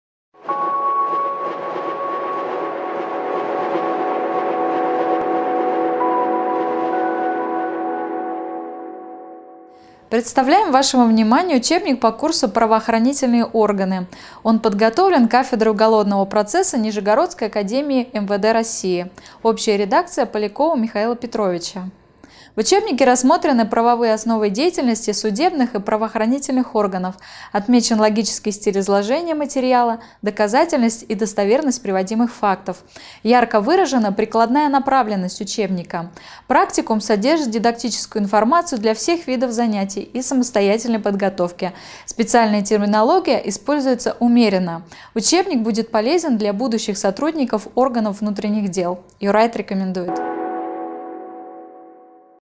"Правоохранительные органы" Под редакцией М. П. Полякова
"Правоохранительные органы"
Учебник и практикум для прикладного бакалавриата

Под редакцией М. П. Полякова